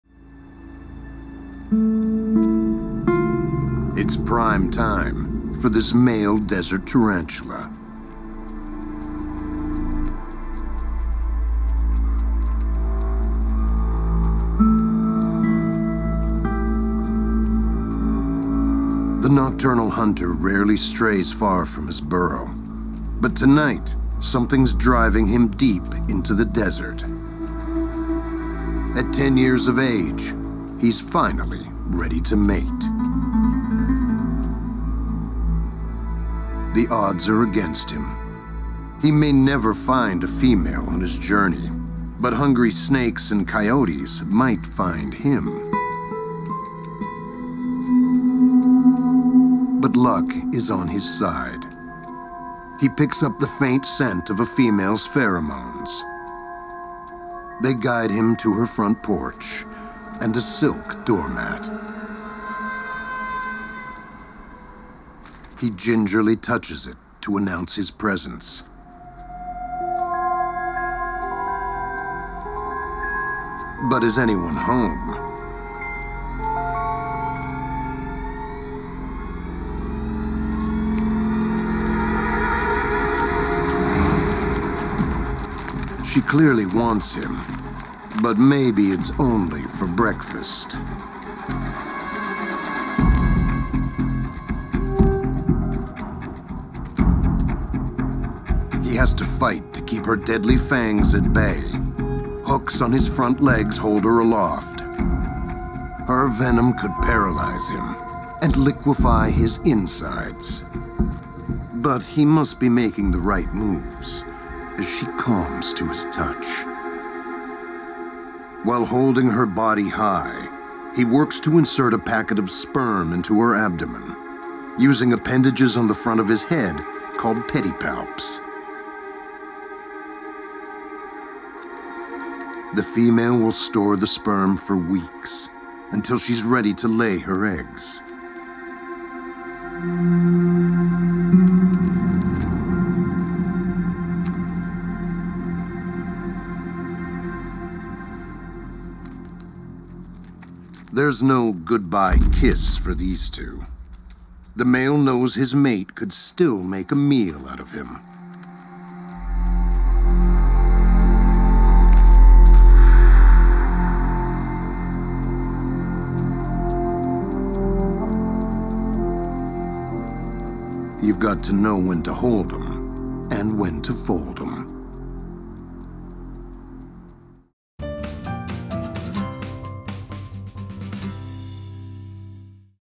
Intense: This Tarantula Mating Ritual Is a Dance w/ Death 🕷️ Smithsonian Channel
For a male tarantula, mating is less an act of love and more a dance with death–especially if the female’s deadly fangs succeed in grabbing him, injecting him with venom, and liquifying his insides.